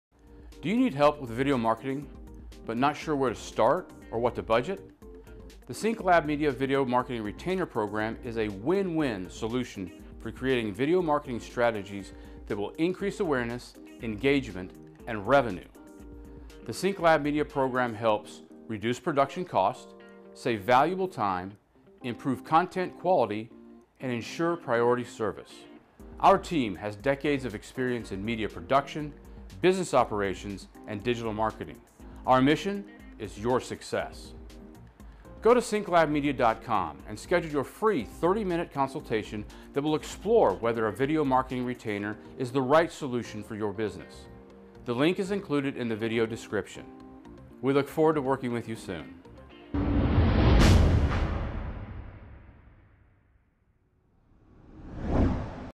Put SyncLab Media on Retainer
Do you need help with video marketing for your business, but not sure how to start or what to budget?

Video marketing retainers are a WIN-WIN solution for creating video marketing strategies and content so that your brand can increase awareness, engagement and revenue.

Video marketing retainers help our clients to:
Reduce Production Cost
Save Valuable Time
Improve Content Quality, and
Receive Priority Service

Our team brings decades of experience in media production, business operations, and digital marketing to the table for our clients. Our mission is your success.

Schedule a time for your free 30 minute consultation to explore whether a video marketing retainer is the right solution for your business. The link is included in the video description.

We look forward to working with you soon.